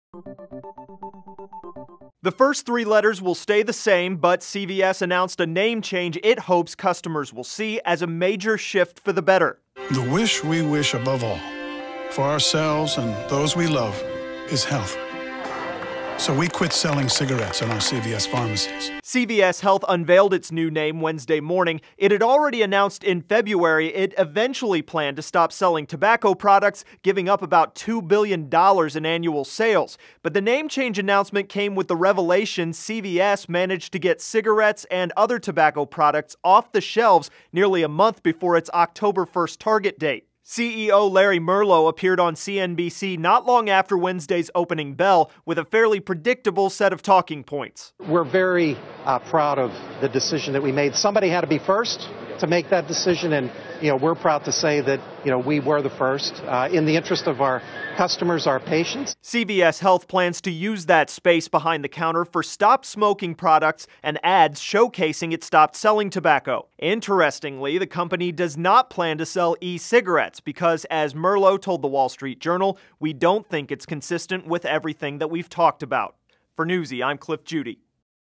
CVS Announces Name Change, Anti-Tobacco Push
CVS' name change to CVS Health also came with the announcement the company's plan to rid its shelves of all tobacco products came a month early.